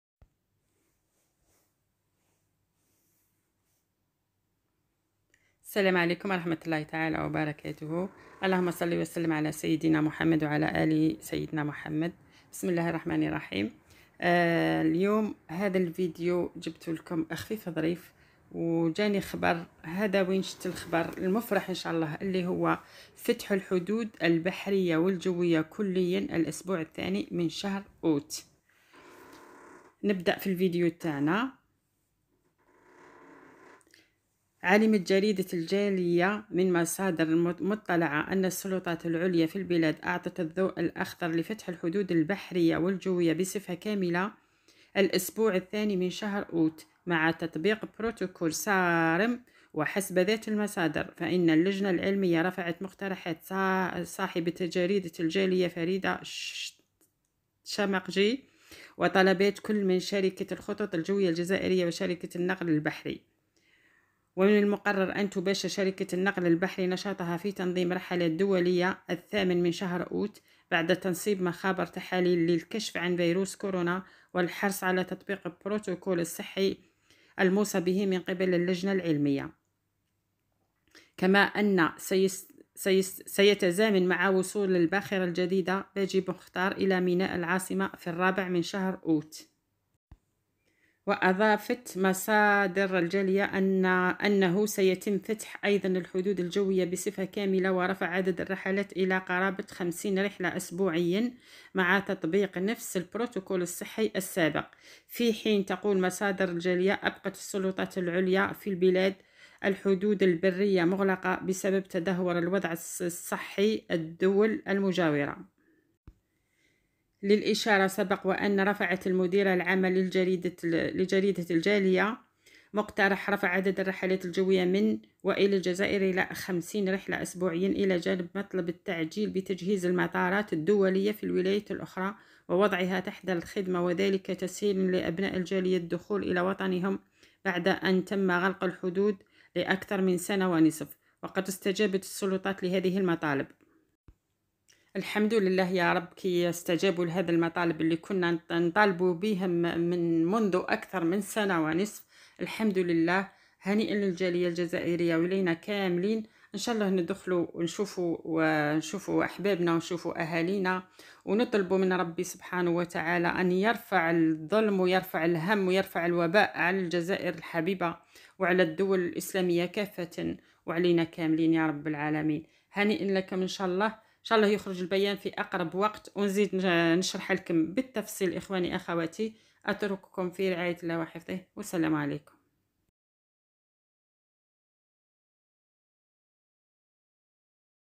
🔴 عاااجل و رسميا / فتح الحدود الجزائرية البحرية و الجوية كليا ابتداءا من هذا التاريخ... 50 رحلة
🔴 عاااجل و مفرح فتح الحدود الجزائرية البحرية و الجوية كليا ابتداءا من هذا التاريخ... 50 رحلة أسبوعيا